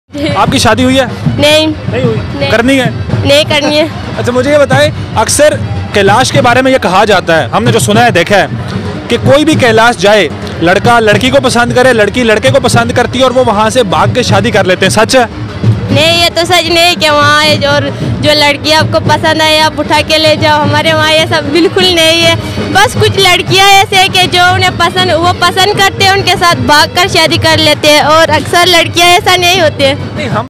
How Kalash Girls Marry In Festival | Talk With Kalashi Girls About Marriage & Culture
In this Video You Will Watch an Exclusive Interview of 2 Kalashi Girls About Their Marriage Culture & Kalash Valley Festival Reality.
This Show is Hosted by Akhi Abbasi